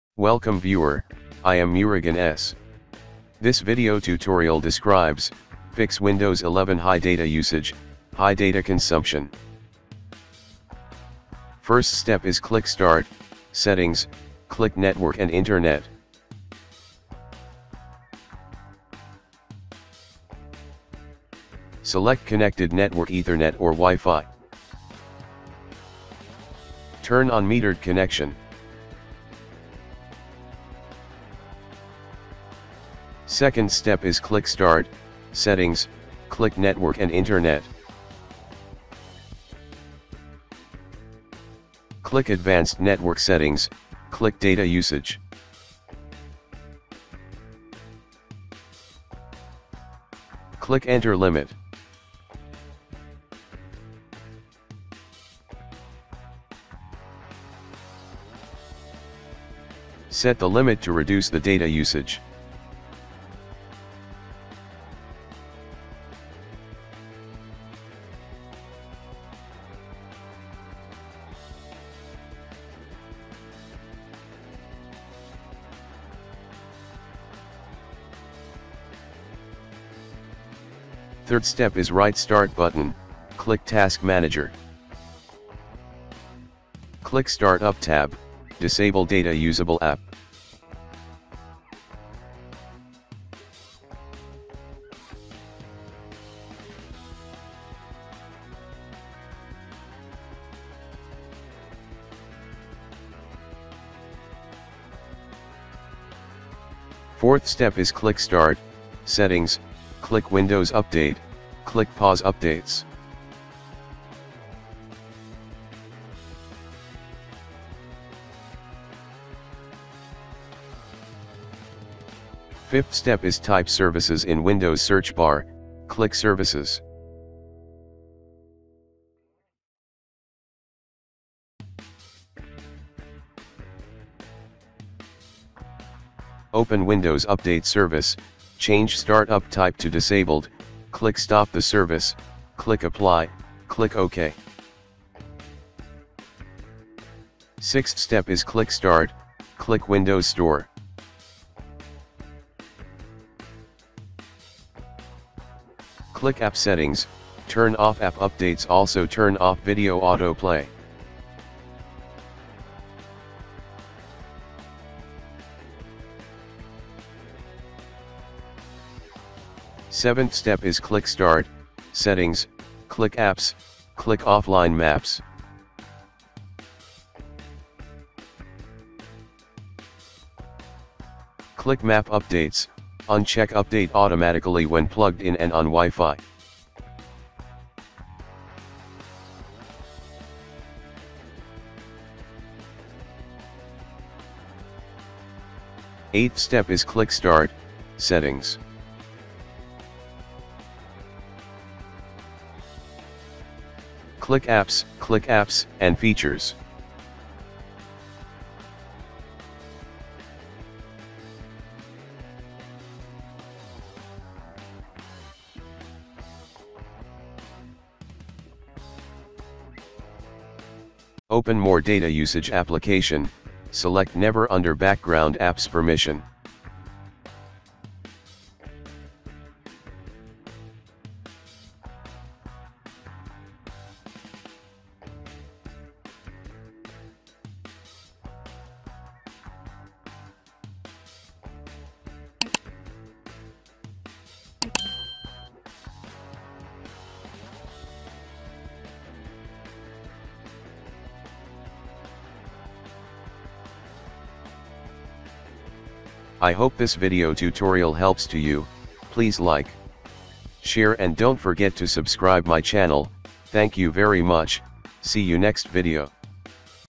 Fix Windows 11 High Data Usage / High Data Consumption / Save Data
This Tutorial Helps to Fix Windows 11 High Data Usage / High Data Consumption / Save Data

00:00 Intro
00:11 Step 1 - Turn on Metered Connection
00:33 Step 2 - Set Data Limit
01:21 Step 3 - Disable Startup Apps
01:46 Step 4 - Disable Windows Updates
02:02 Step 5 - Disable Windows Update Service
02:27 Step 6 - Change Windows Store App Settings
02:50 Step 7 - Disable Map Updates
03:16 Step 8 - Change Background Apps Permissions
04:16 Closing